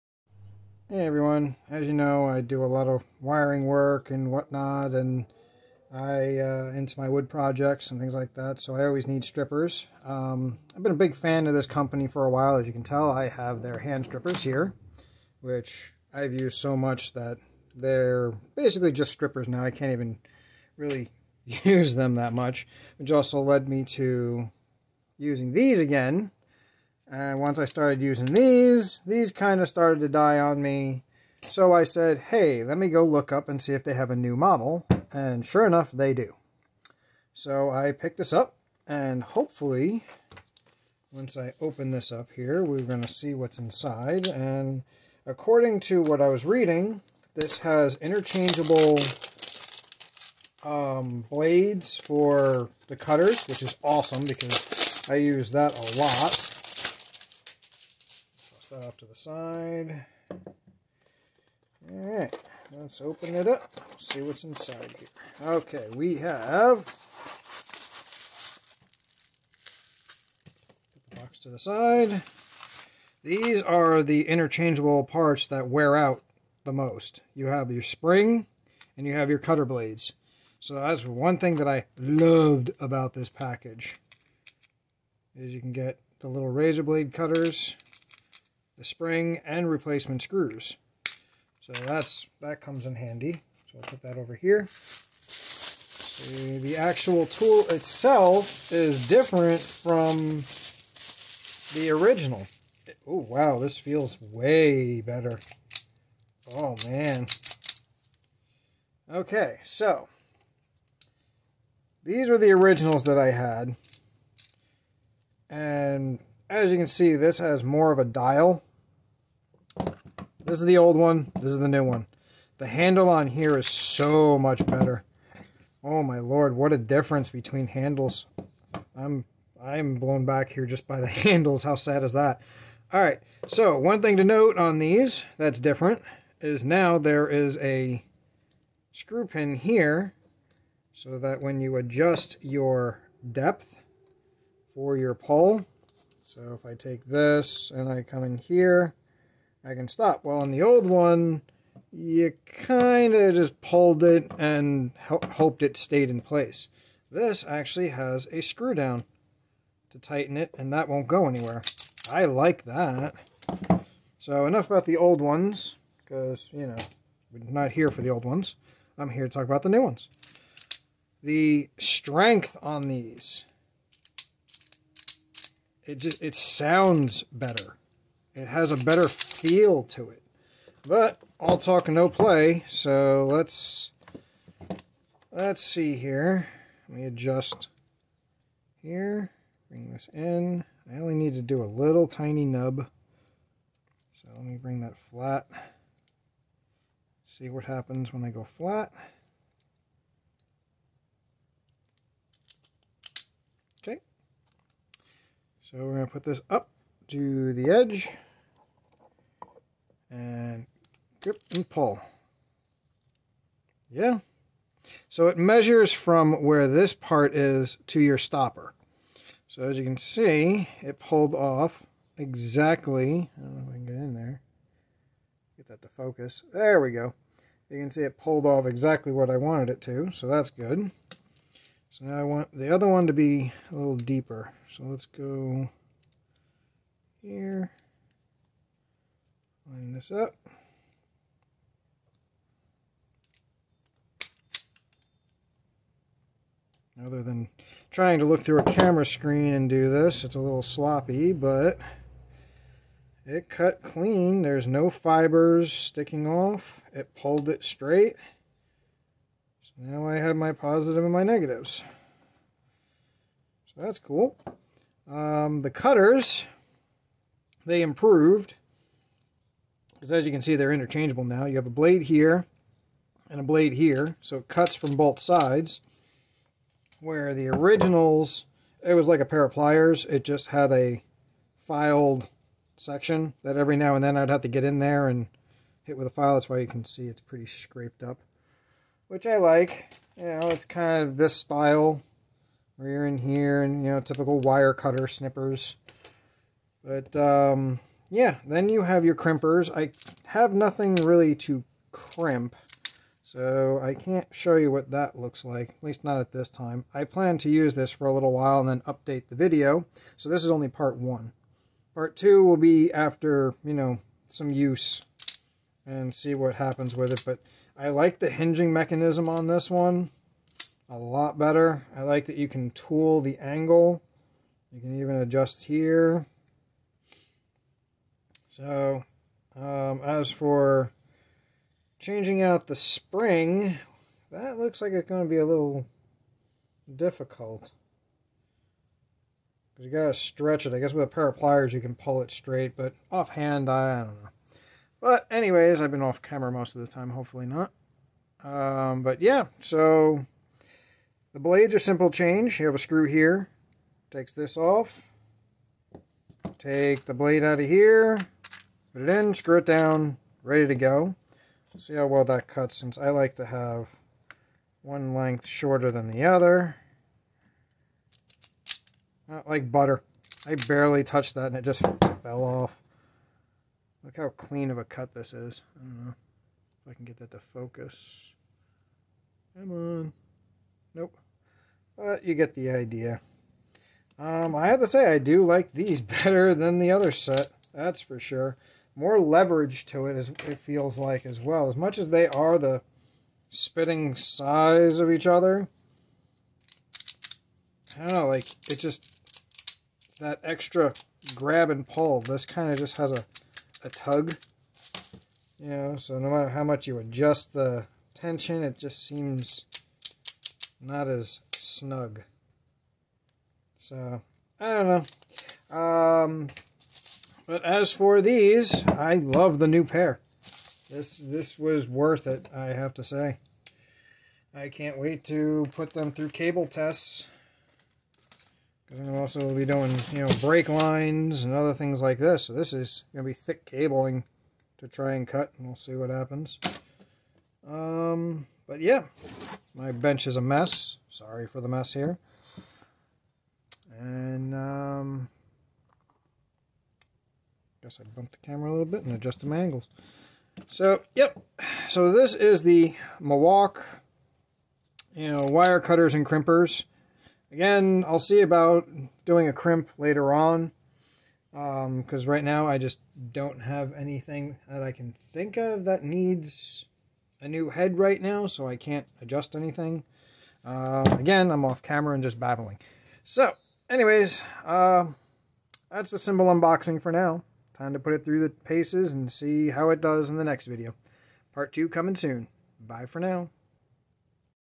MulWark 3 in 1 Automatic Self Adjusting Wire Stripper
Quick Unboxing and comparing the older version to this new one. I was in the market for  new one and loved this brands version and when i saw they had a new one i just had to have it. world of difference, better quality to it and more accurate for repeatable cuts. The new blade cutter works sooo much better. All in all I like it and cant wait to put it through its paces the old one lasted me many years before i had to sharpen it and mess with the auto cut.